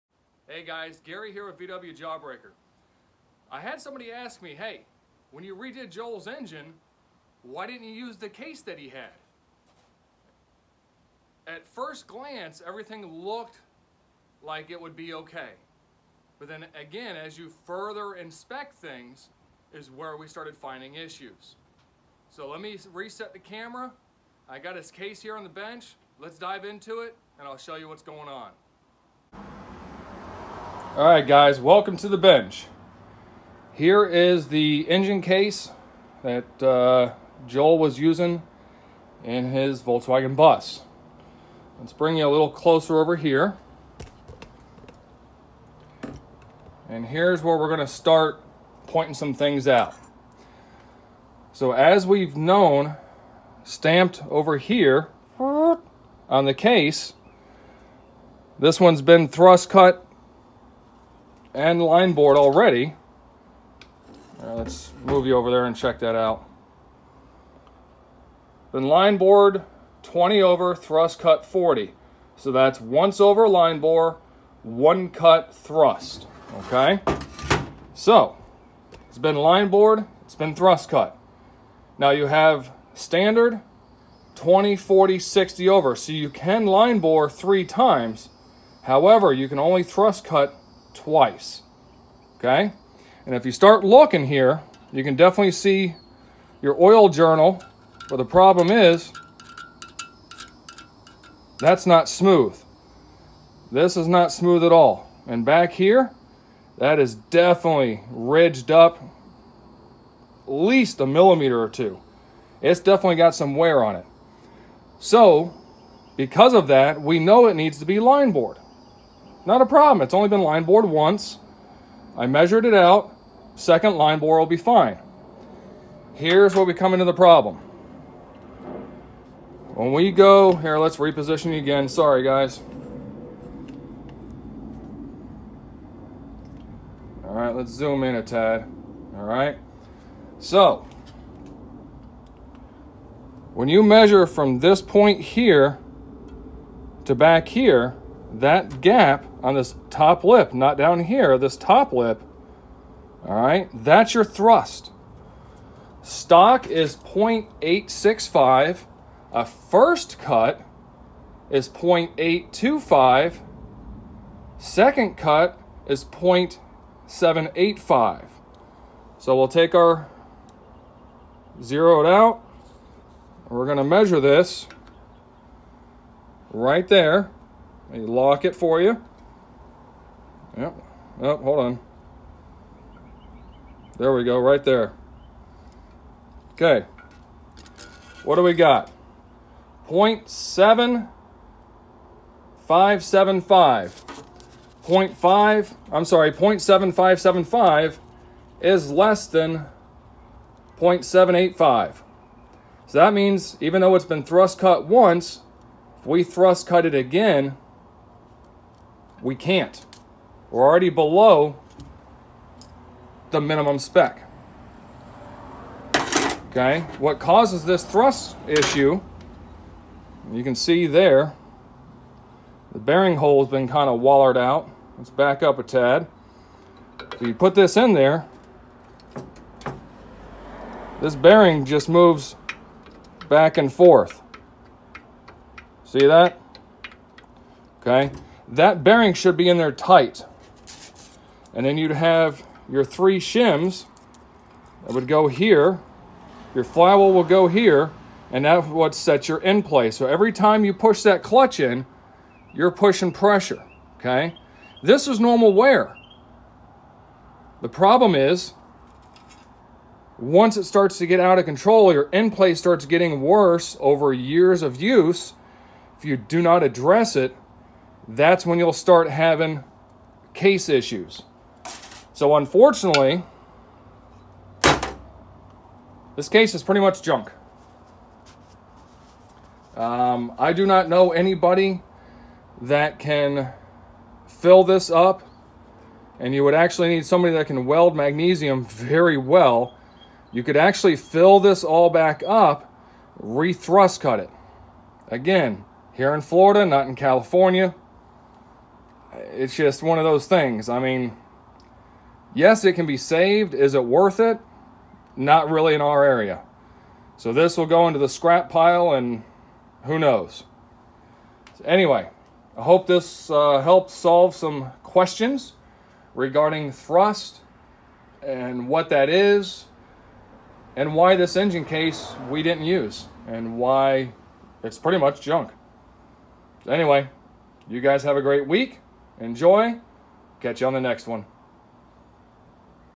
AirCooled VW Engine Case Thrust Surface and Problems
We are taking a look at why we couldn't use the engine case from Joel's bus.

I enjoy reading your comments and respond to them all individually!

The online VW commuity is awesome, thanks to people like you! 👍